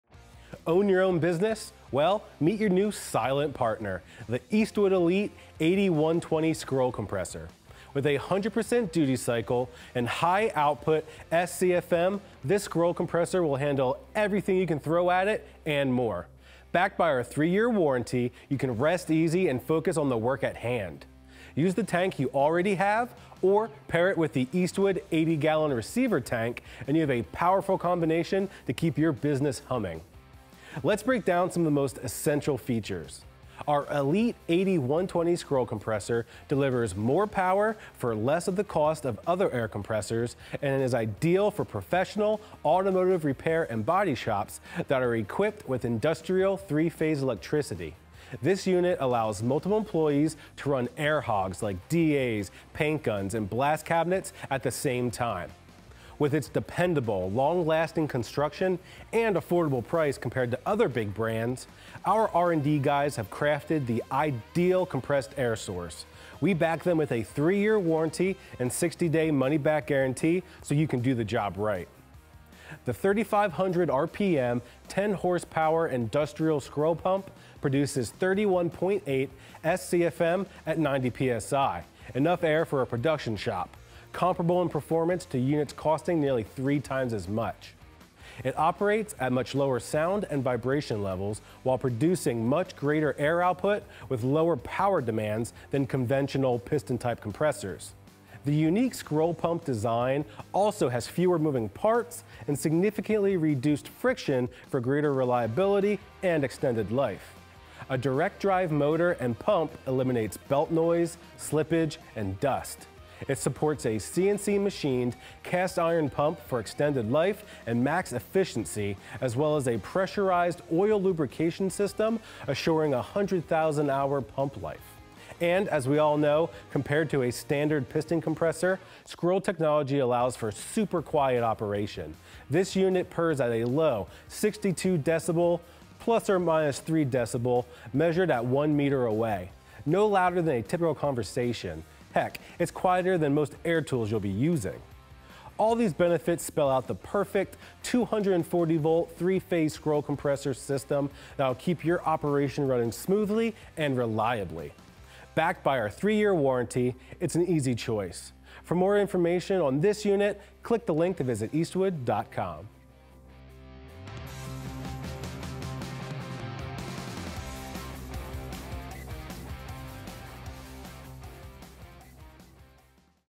The PERFECT Shop Compressor? ELITE QST™ 80/120 SCROLL AIR COMPRESSOR: Quiet. Powerful. Affordable!
Put your right foot forward and equip your shop with a scroll compressor that can keep up with even the most demanding workflow WITHOUT the racket of a loud piston compressor!

EASTWOOD ELITE QST™ 80/120 SCROLL AIR COMPRESSOR
ITEM #32500

DELIVERS MORE POWER FOR LESS THE COST OF OTHER AIR COMPRESSORS
Our Elite 80/120 Scroll Compressor is ideal for the professional automotive repair and body shops . With its dependable, long-lasting construction and affordable price compared to other big brands, our R&D guys have crafted the ideal compressed air source. Read more about the advantages of scroll compressors below or call our experts. We back them with a three-year warranty and 60-day money-back guarantee so you can Do the Job Right.

SCROLL AIR COMPRESSOR FEATURES:
Innovative - Direct Drive Cast Iron Scroll Pump
Integrated Air Cooler - Multi-stage oil filtration system
Low Noise Level - Quiet Scroll Technology and only emits 62DB +/- 3Bd
Powerful - 10 HP Motor delivers 31.8 SCFM @ 90 PSI Air Output
Dependability - 100,000 hour pump life, with 100% Duty cycle